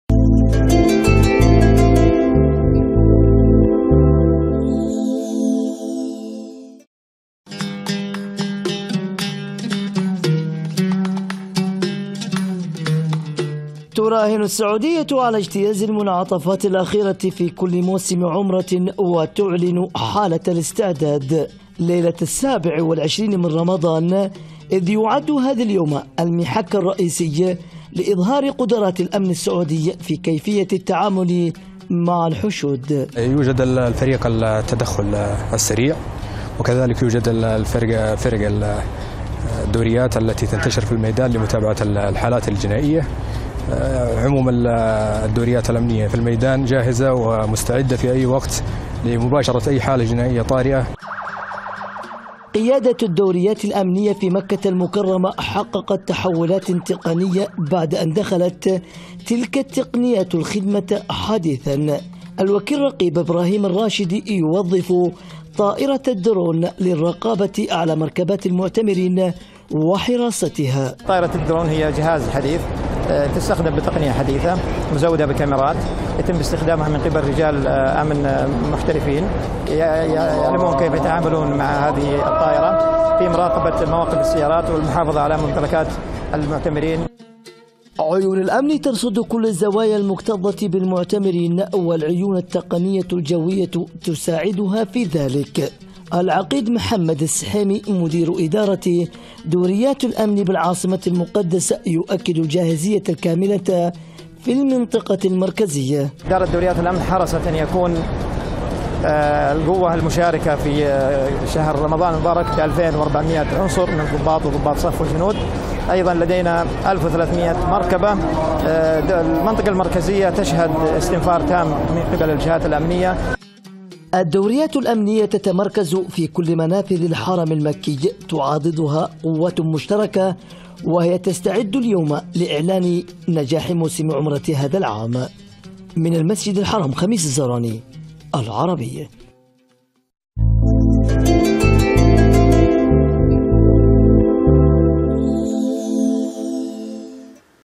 وجوه من الحرمين | حال أمن الحرم مع قرب وداع رمضان
أكدت قيادة الأمن العام في السعودية جاهزيتها في إدارة الحشود، في ليلة السابع والعشرين من رمضان وبات المسجد الحرام يشهد كثافة فى عدد الزوار، قد تفوق المليوني مصل.